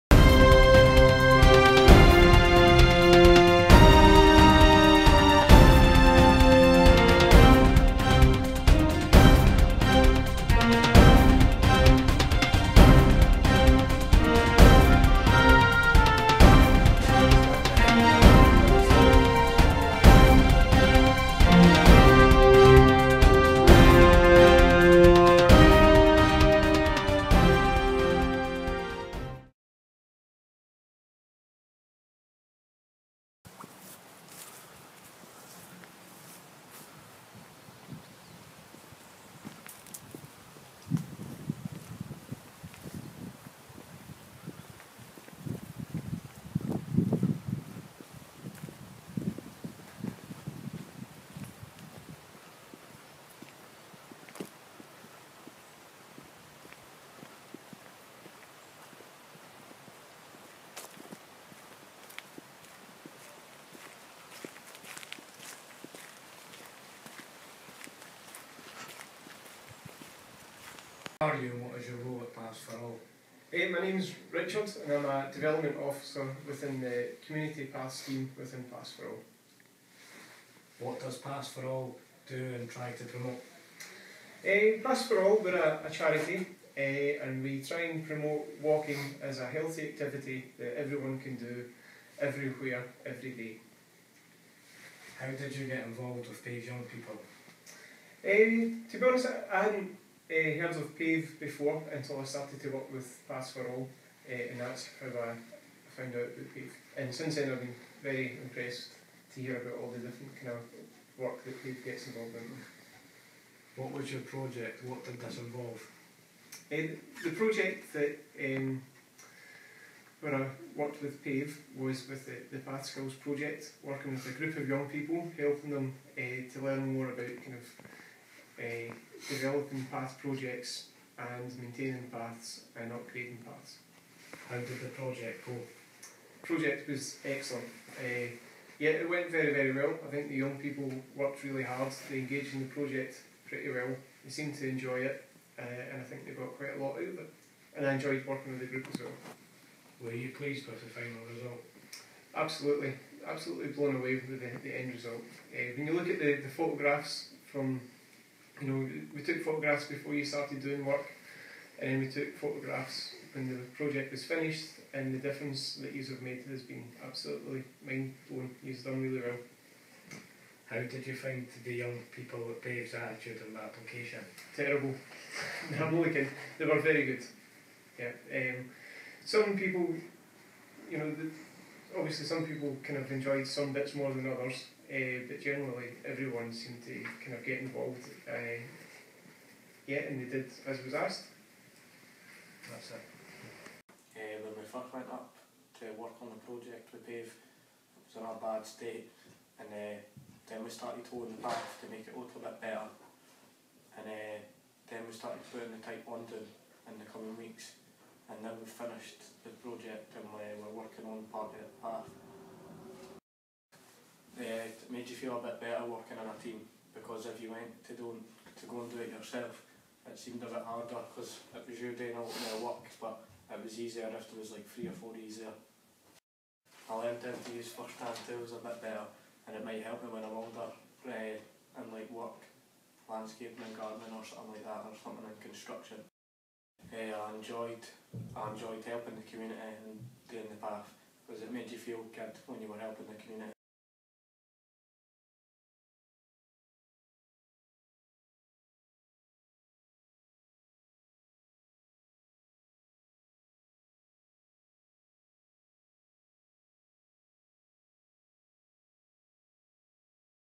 Path skillz news
A film made in collaboration with the PAVE pupils who took part in one of our Path Skillz projects.